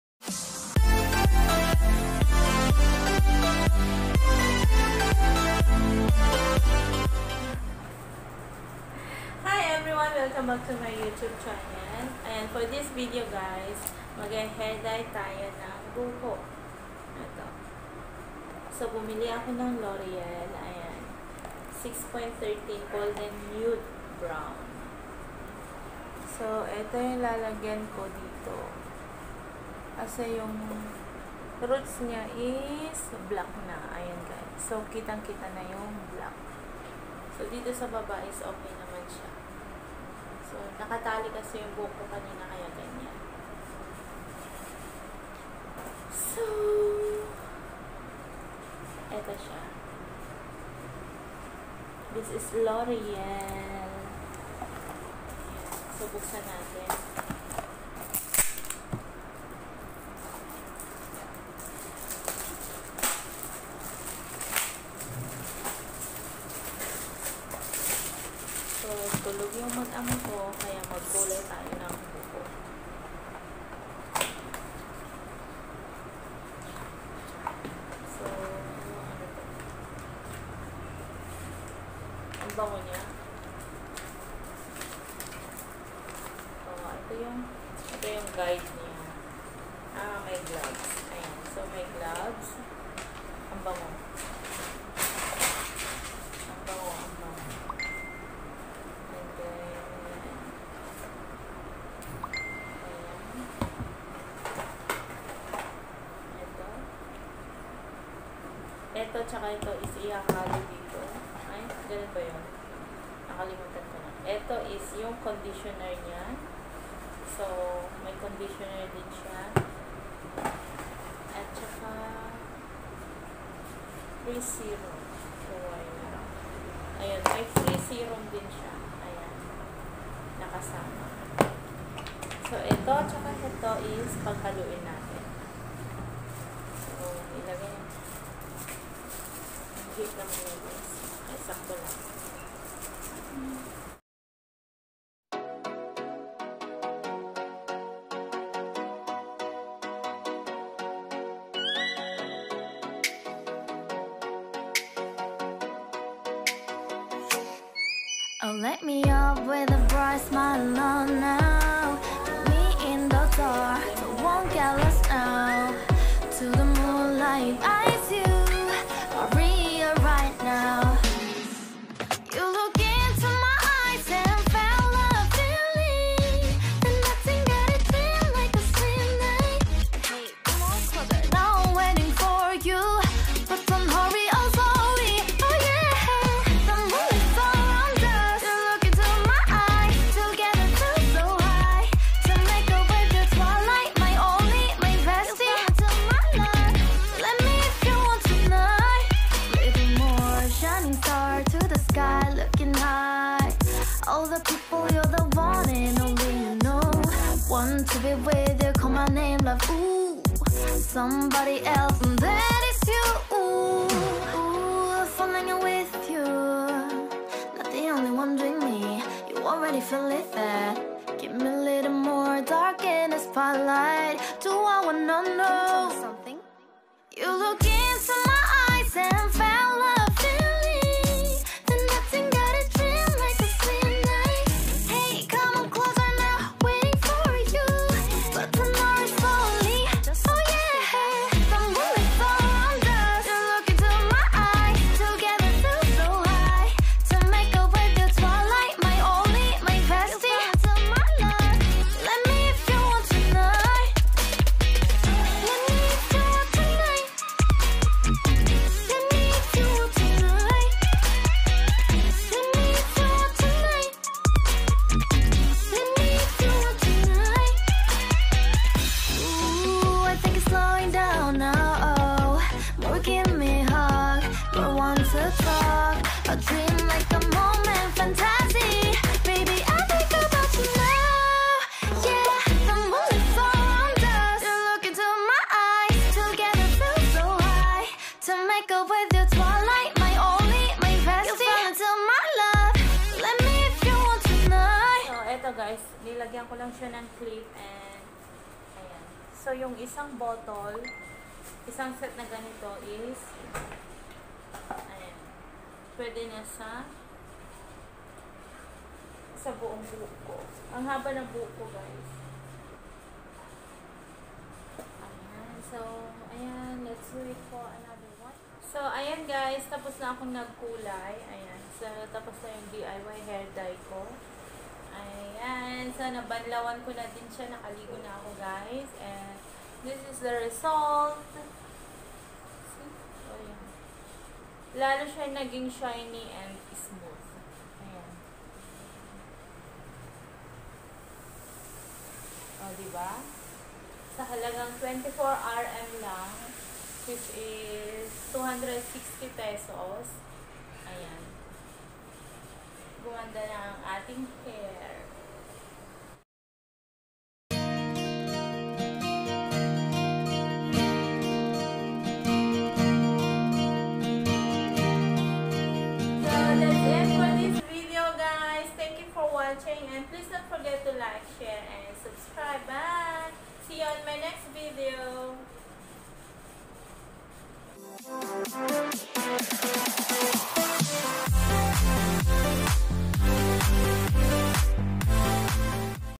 DIY Hair Dye (L'Oreal) | MayLhyn Vlogs
Let's do DIY hair dye nagtitipid eh, hehehe!